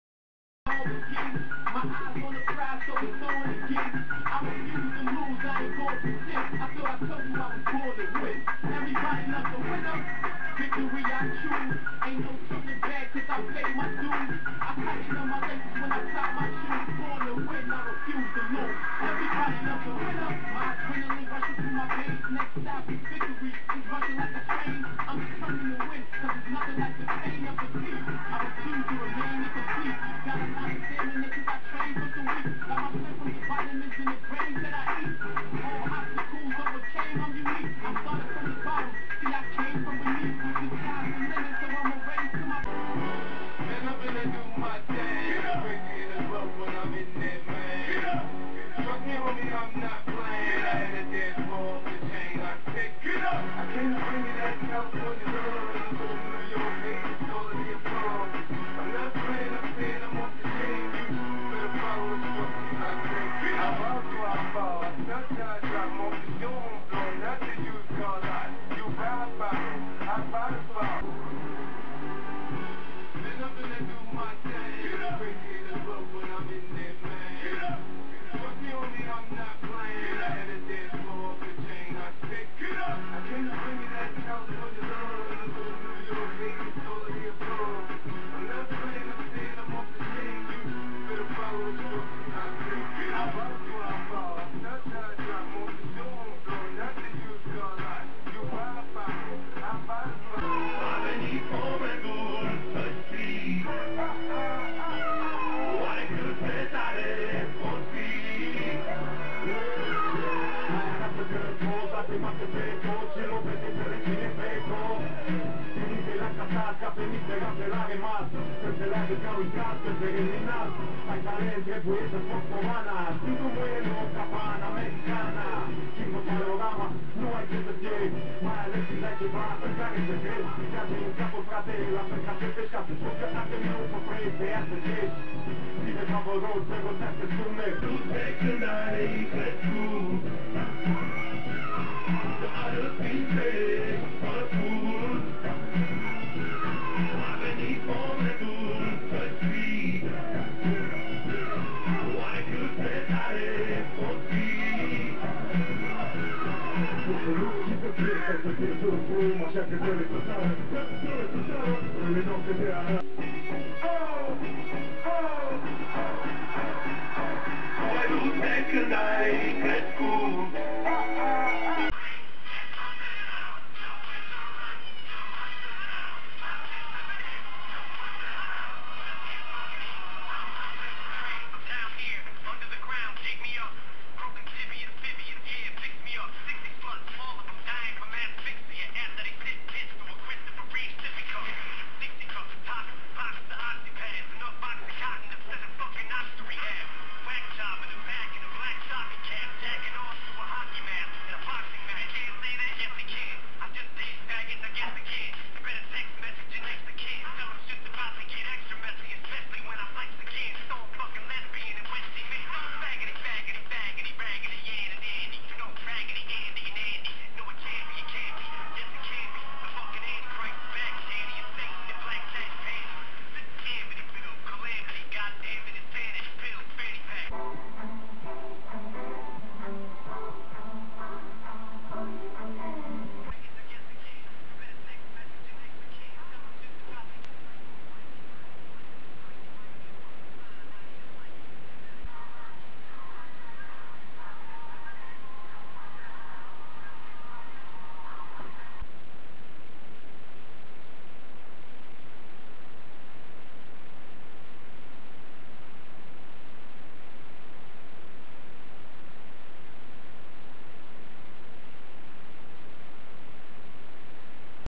Training for planche - closer than ever
I am training for the planche for some time and i fell like i am close than ever, i am able to hold a bent arms planche for some time but i want a perefect form straddle planche!
I workout every monday,wednesday, friday and i train parkour and acrobatics in weekends.
If you want my workout program here it is:
Tucked planche push-ups: 3 sets, max rep each set
Bed dips (with no space between palms, they are touching- hand placement like in diamond pushups) leg far apart from the bed): 3 sets, 1st-30 reps, 2 &3 20 reps
extra wide pushups: 3 sets, max rep every set, after you can no longer perform a correct push ups you get down on your knees and continue doing them till you drop - you do this every set.